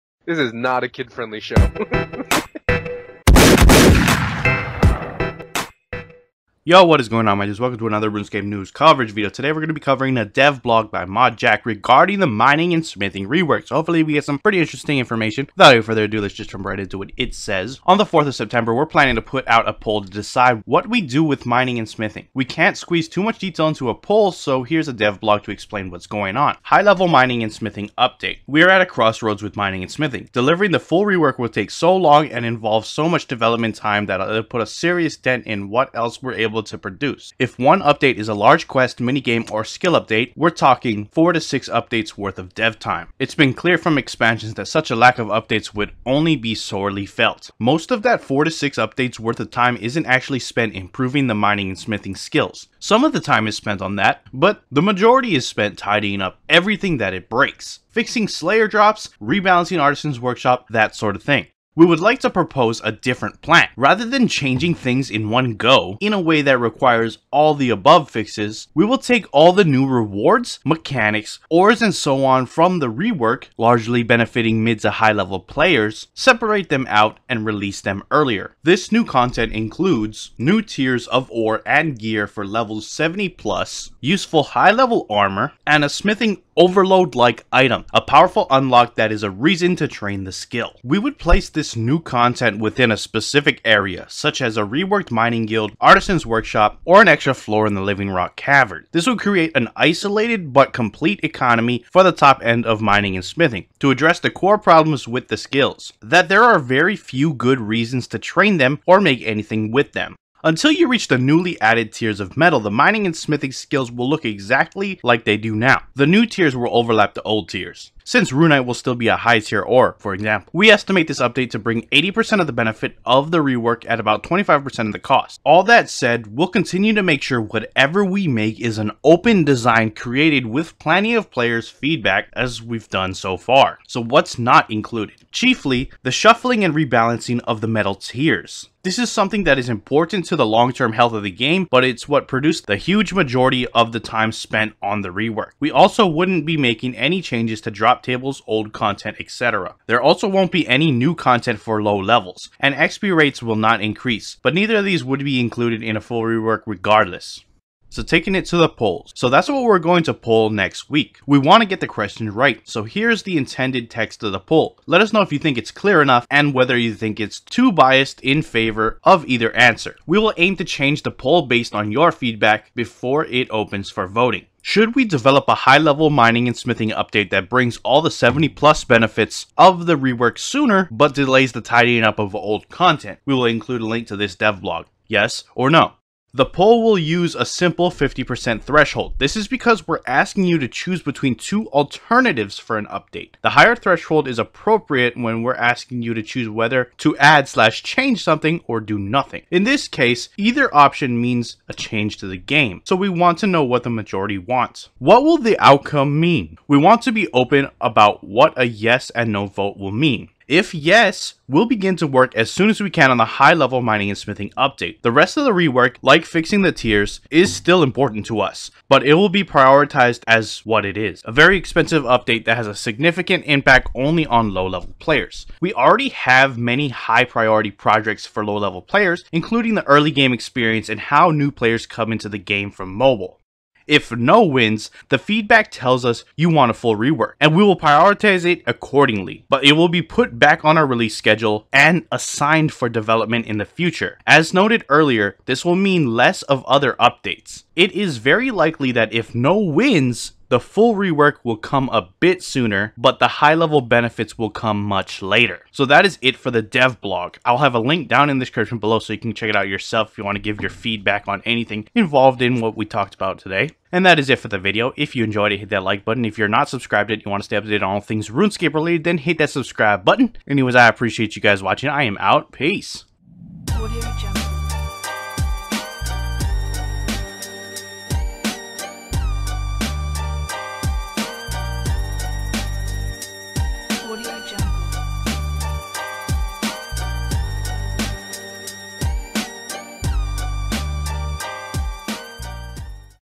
High-level Mining & Smithing - Dev blog
Mod Jack goes over a poll that will be going out soon asking the players what direction we want Jagex to take in terms of the Mining & Smithing rework! Hope you enjoy!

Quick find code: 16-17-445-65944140